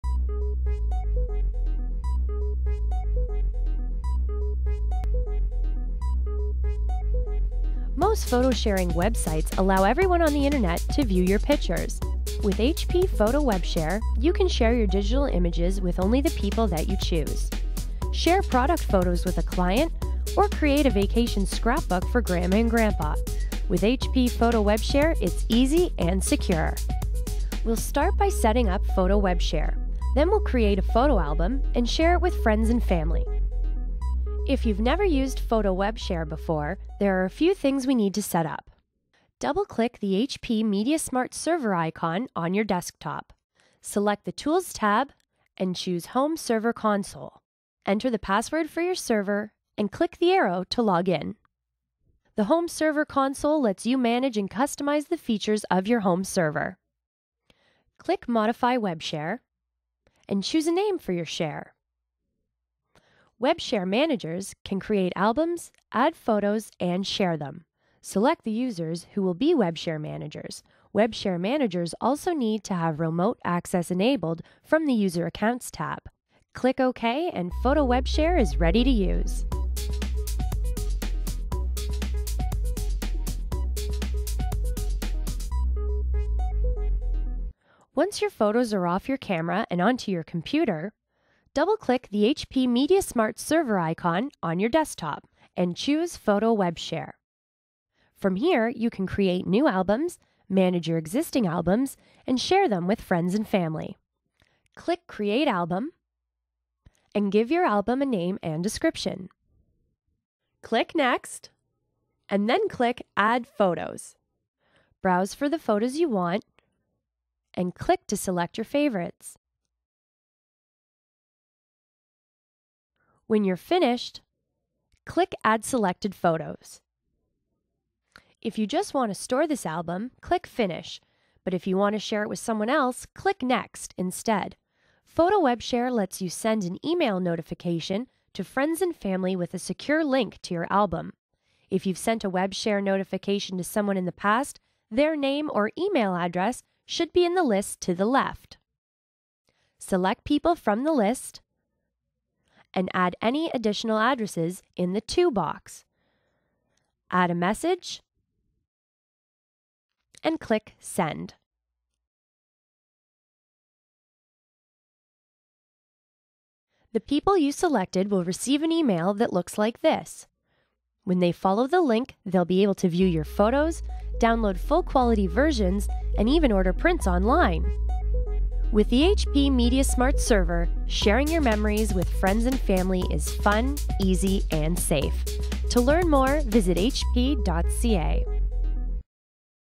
HP MediaSmart Server - Using HP Photo WebShare
How to set up your server to share photos with friends and family using HP Photo WebShare